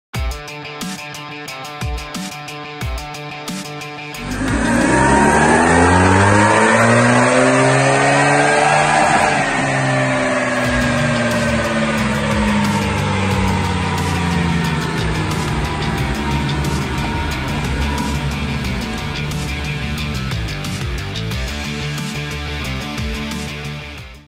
BMW R nineT with Custom BrenTuning Flash on the Dyno
As many of you know, we work on A LOT of BMW's.  We picked up some good power with our custom "R nineT" flash!  Condidering the stock baseline was 99whp, making 9whp above that with no changes except our custom tune is amazing.  We also had a lot of complaints about the throtte response these bikes have stock, so we improved that as well.  This bike now feels how it should have from the factory.

This custom flash is avaiable for all BMW 1200 motors, air and water cooled.